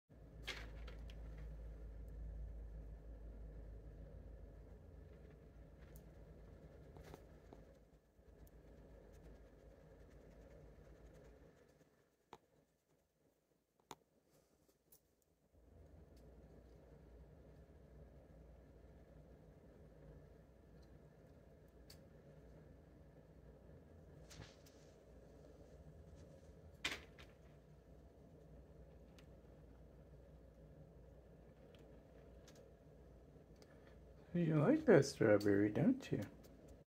Cute! Roof Rat eating a strawberry. Rats love Fruit, and it's good for them!
My pet roof rats love Strawberries.  Anika is happy to demonstrate. :-)

If you would like to get a pet rat like Anika, please get in touch: